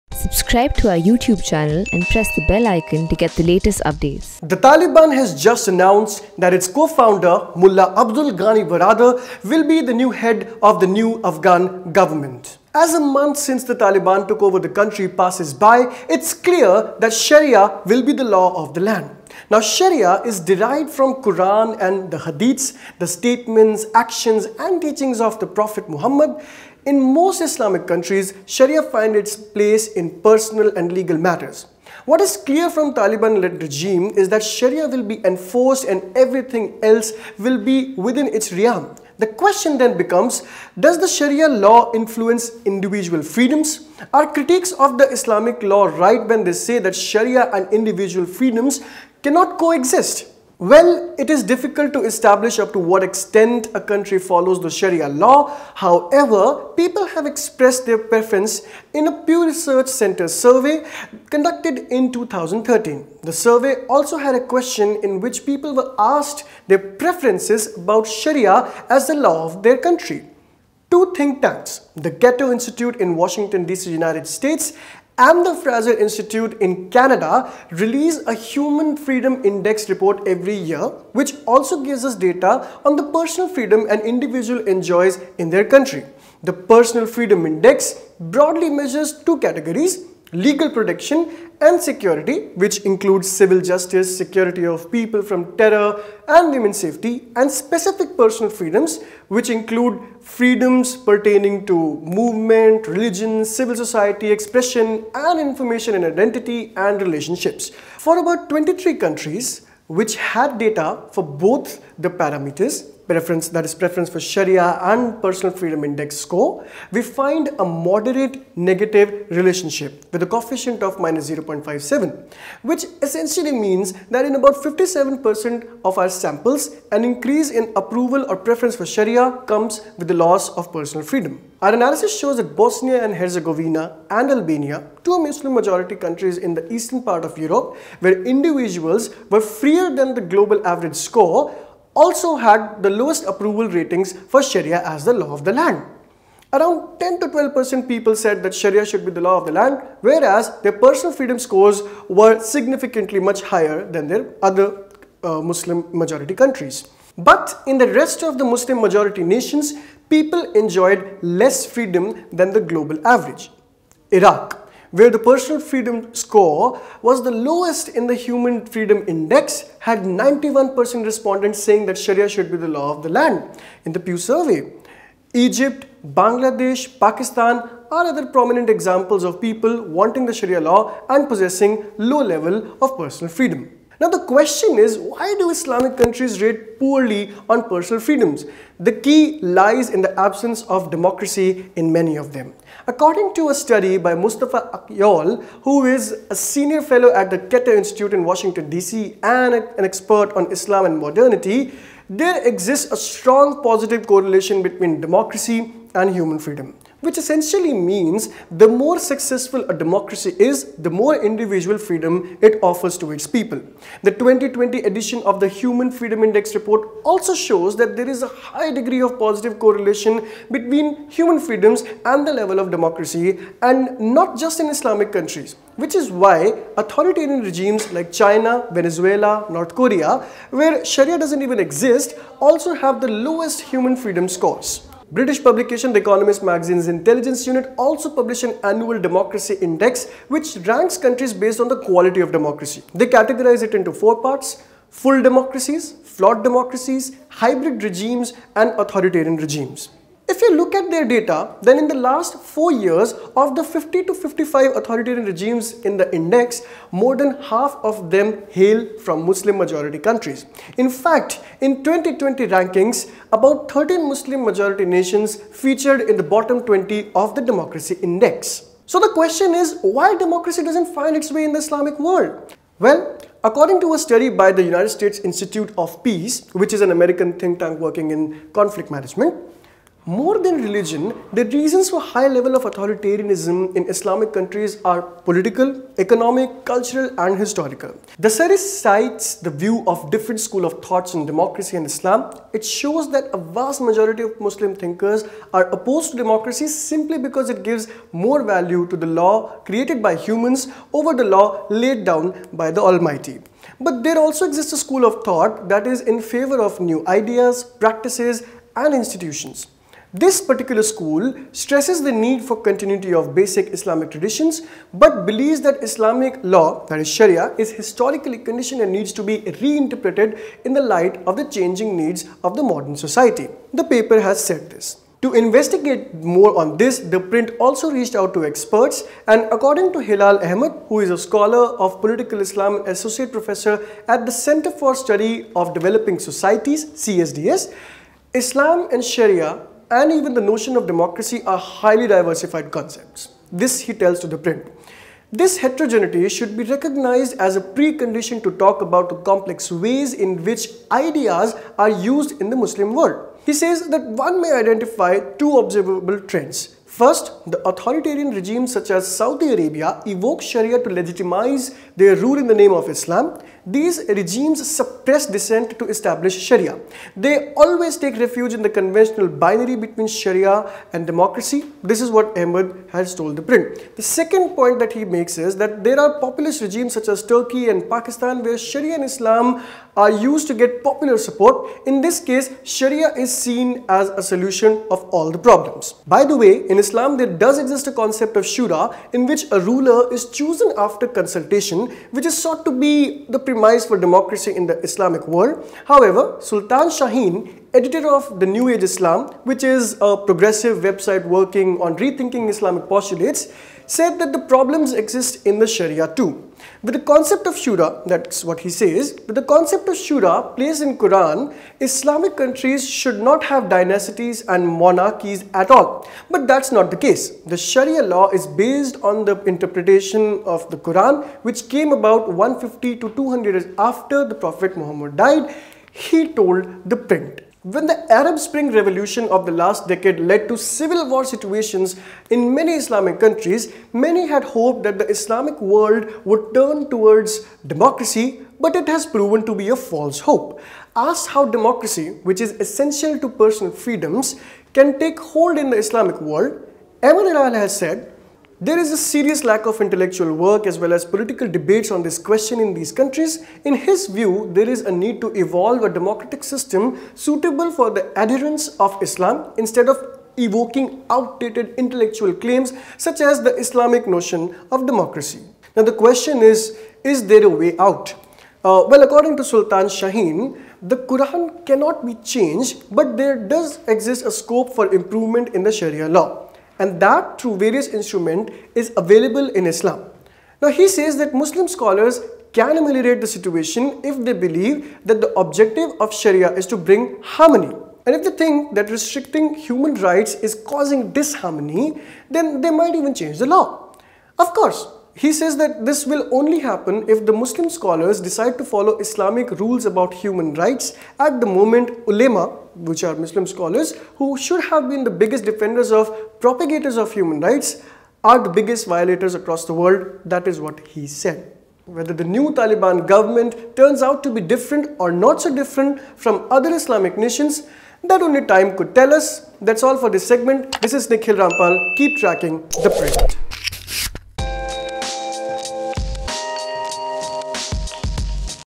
What data tells us about Sharia law & personal freedom in Islamic nations
Data points to a moderate negative relationship between preference for Sharia and the freedom an individual enjoys in Islamic nations. Most Muslim majority nations also account for more than half of the world's authoritarian regimes. Nikhil Rampal explains how, in this video.